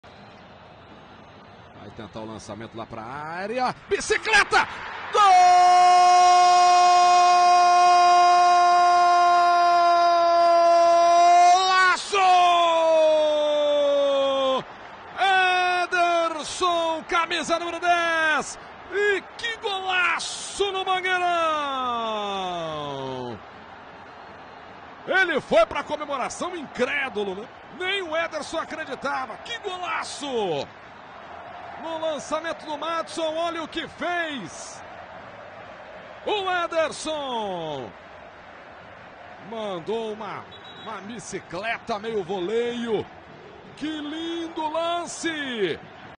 GOLAÇO Éderson - Vasco 1 x 0 Paysandu - BRASILEIRO SÉRIE B 04/10/2016
Gol Éderson - Vasco - Brasileiro Série B 2016 04/10/2016

Golaço de Éderson! O atacante recebe cruzamento de Madson e acerta um lindo voleio para abrir o placar!